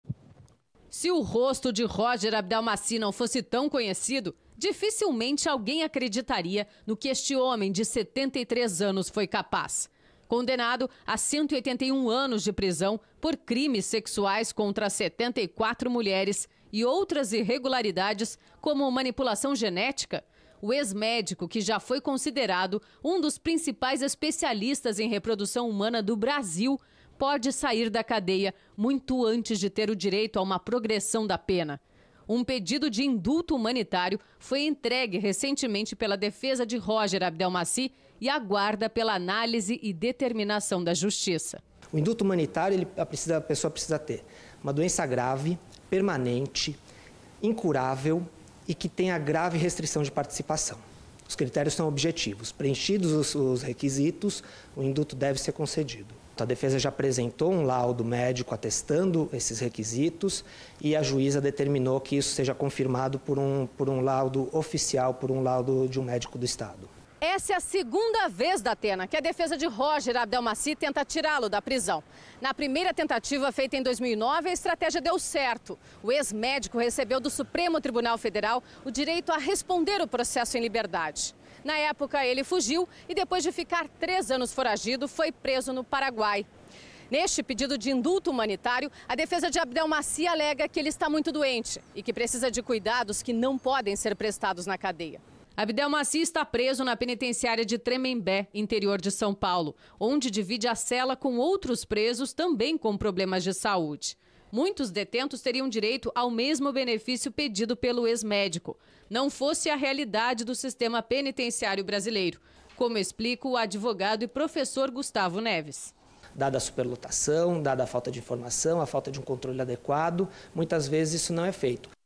Médico estuprador entra com pedido de liberdade
O médico foi condenado por atacar e estuprar 74 pacientes. Condenado a mais de 100 anos de prisão, o homem entrou com pedido de indulto humanitário para conseguir se livrar do restante da pena.